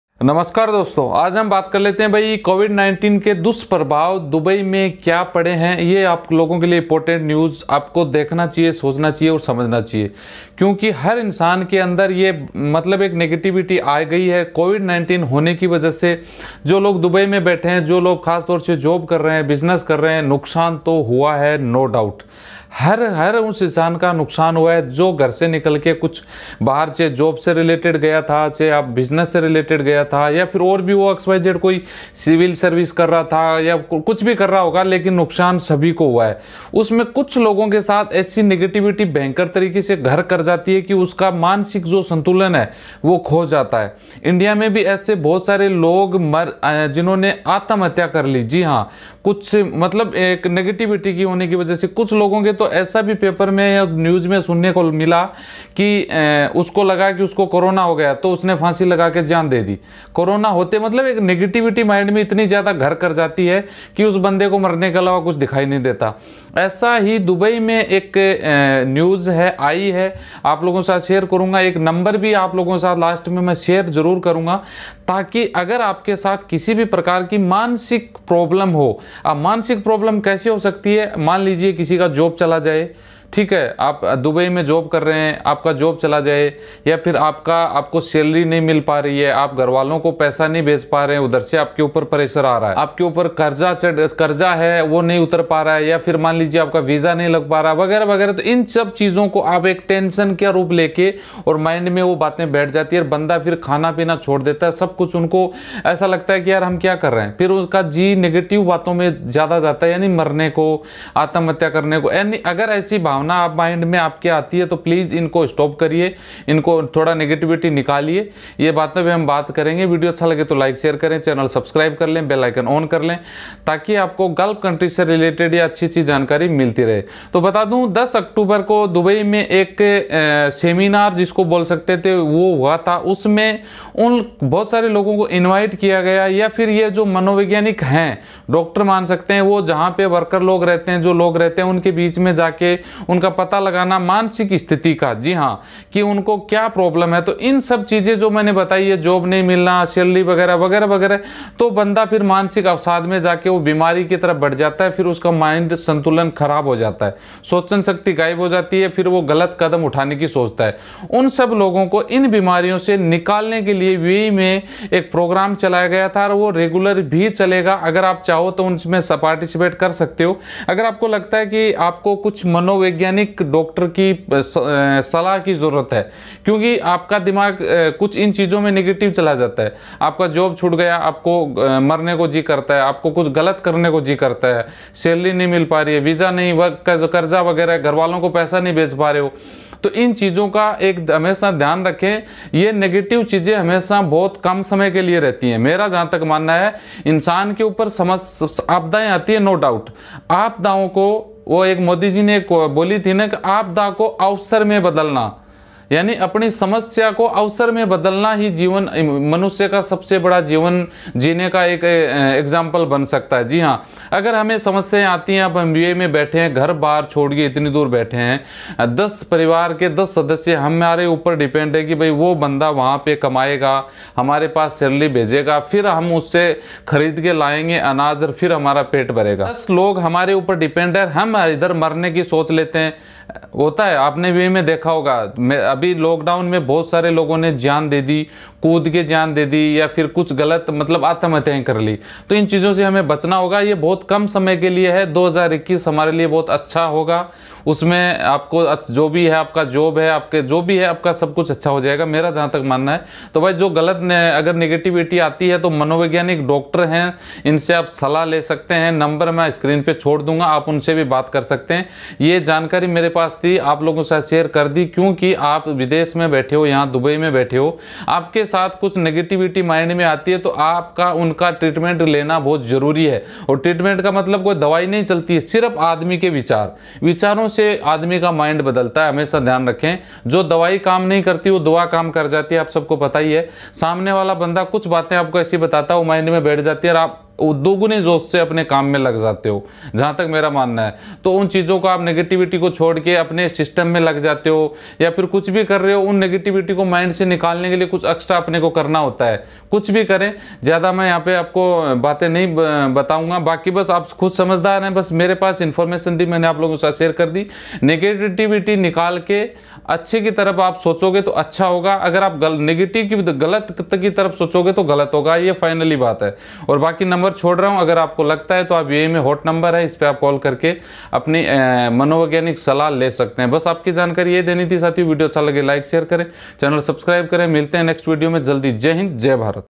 इस कोरोना काल में जॉब वर्कर को घबराना नहीं चाहिए।मानसिक तनाव से मुक्ति पाने के डाक्टर ने परामर्श ले।
Account-Ashok Kumar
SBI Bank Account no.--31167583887
IFSC Code-SBIN0031123